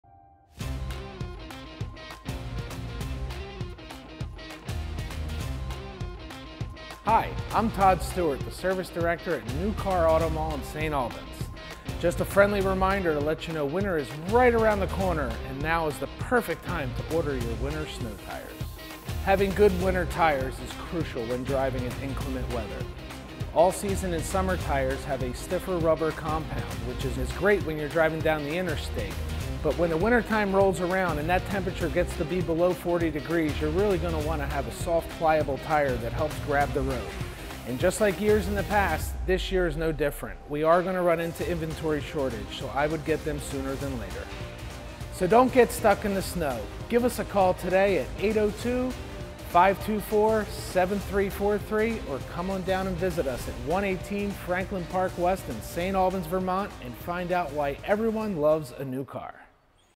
Winter is Coming Don't Wait to Get Your Snow Tires!
With winter right around the corner, don't wait until you're stuck on the side of the road to get your snow tires.